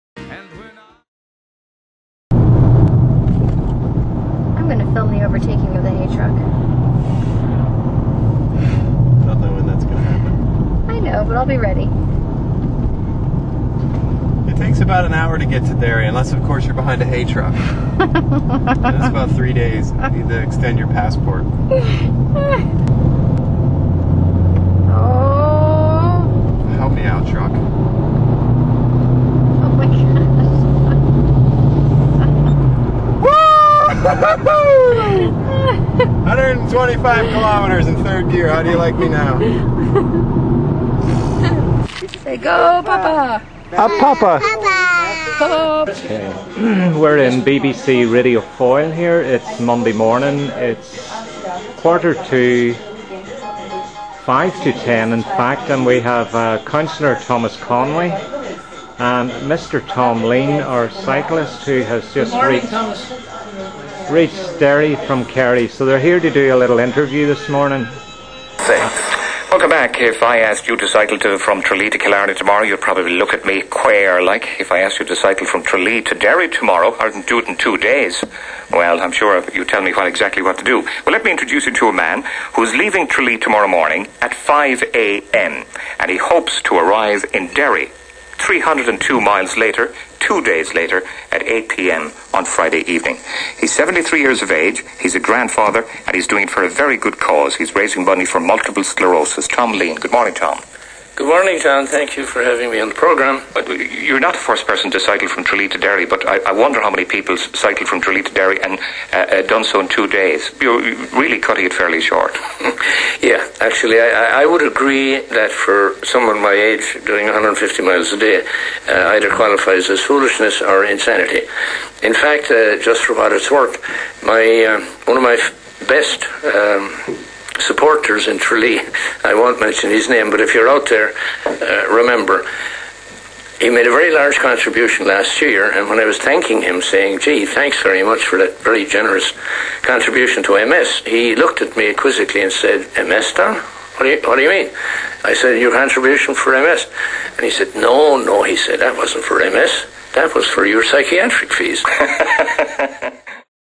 Kerry to Derry Ride for MS
My father-in-law rides his bike in Ireland to raise money for MS. He does 320 miles in two days. Over his life he's raised nearly $200,000 US. Amazing.